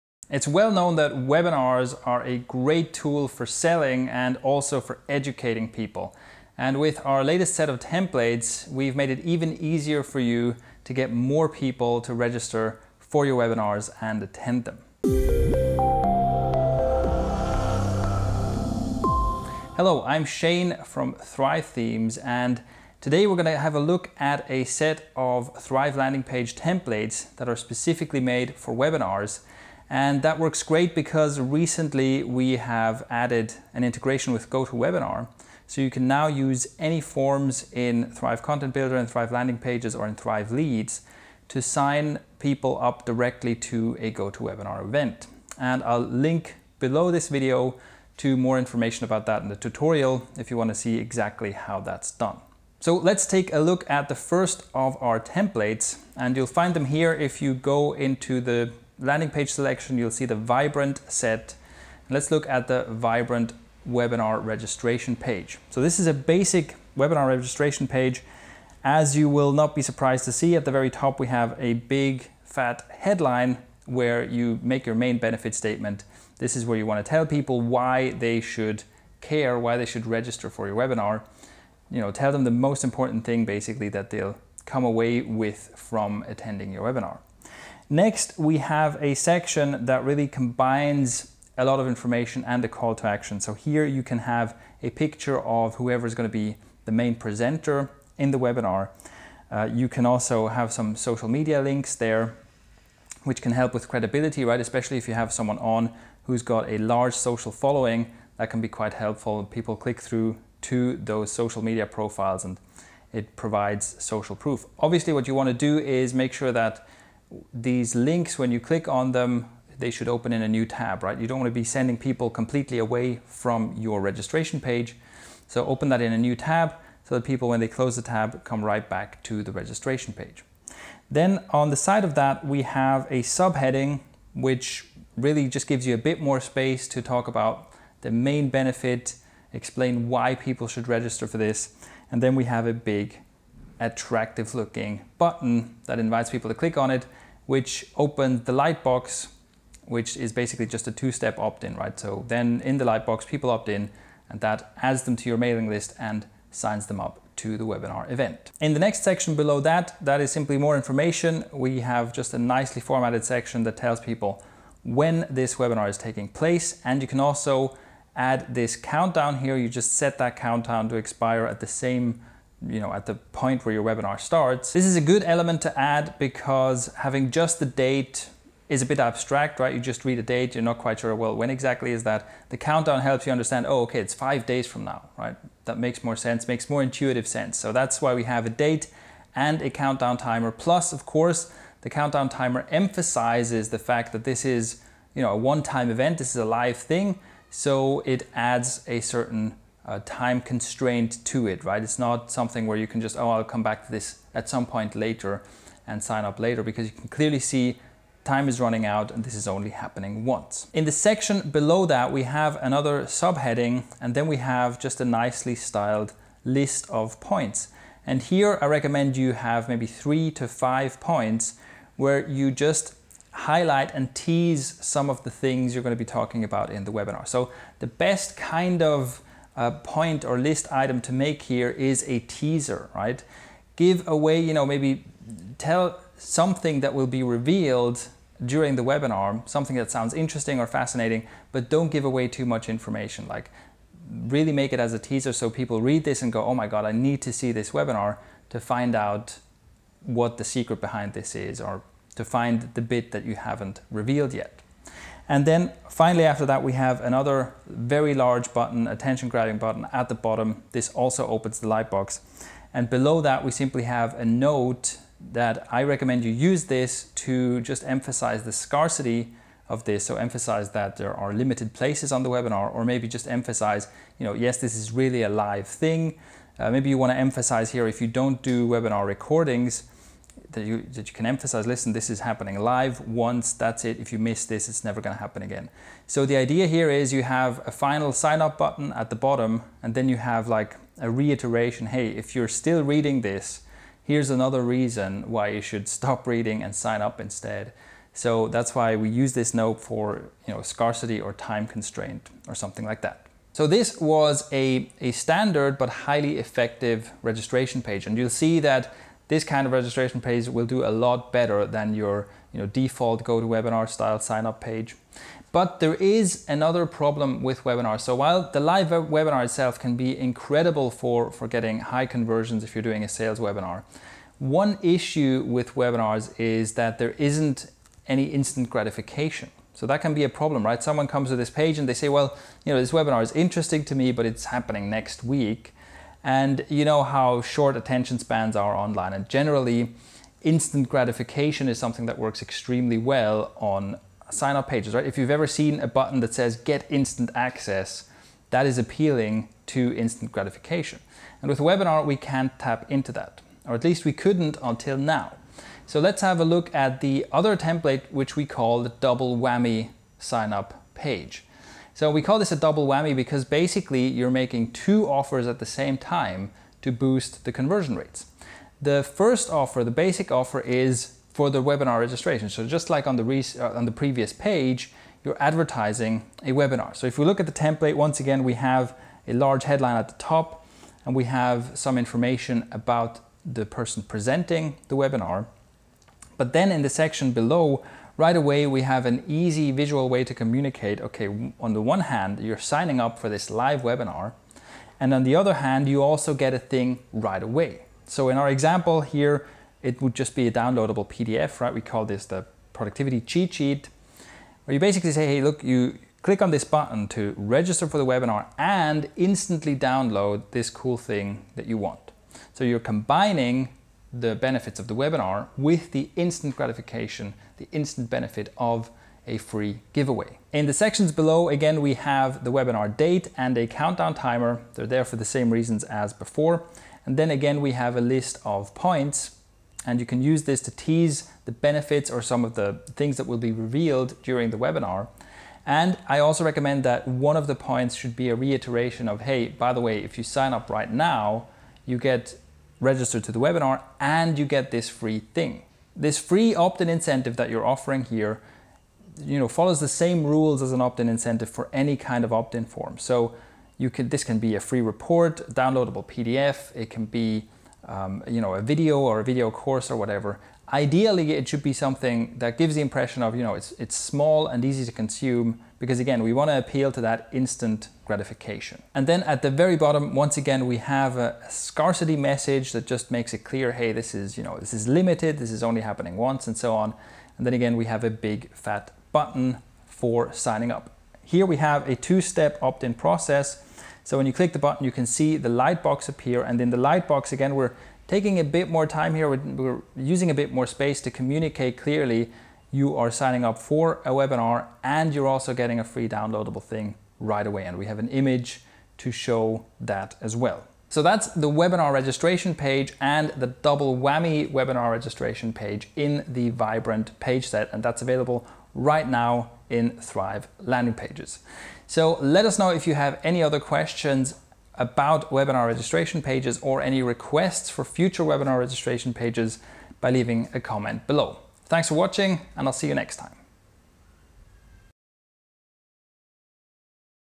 Build a Landing Page to Get More Webinar Attendees
A look at 2 new webinar registration templates that employ some clever tricks to help you get higher signup conversion rates.